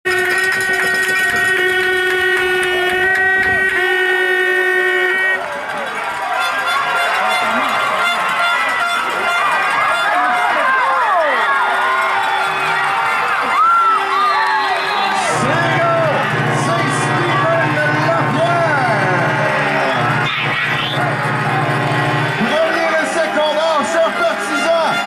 Course de 63 verges du # 35 Steven Lapierre des Condors (vs Triades) 08/10/2016
Course de 63 verges du # 35 Steven Lapierre des Condors du Cégep Beauce-Appalaches (vs Triades de Lanaudière) 08/10/2016